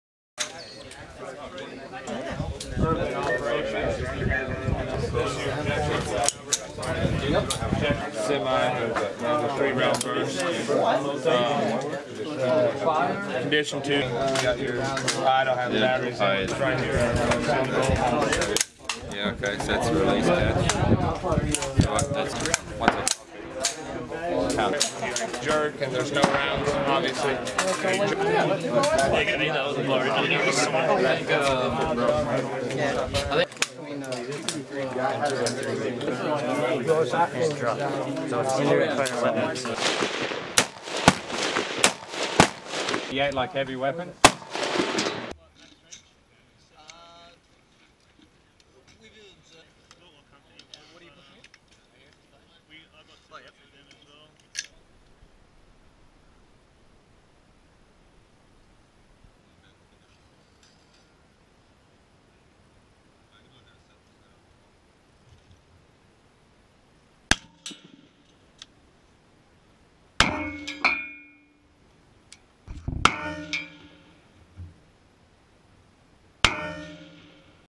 US Marines & Australian Defense Force Battle Sight Zero (BZO) Weapons
ROBERTSON BARRACKS, Northern Territory, Australia - Marines with Alpha Company, 1st Battalion, 5th Marines, Marine Rotational Force-Darwin hike to the Robertson Barracks Training Area range to BZO their rifles as well as exchange information with Australian Defence Force Soldiers about weaponry used by Alpha Company. MRF-D enhances interoperability with the ADF and strengthens the operational fluidity of both parties. Video by Cpl. Scott Reel | Marine Rotational Force Darwin

AiirSource - A video and news hub for aviation/military enthusiasts.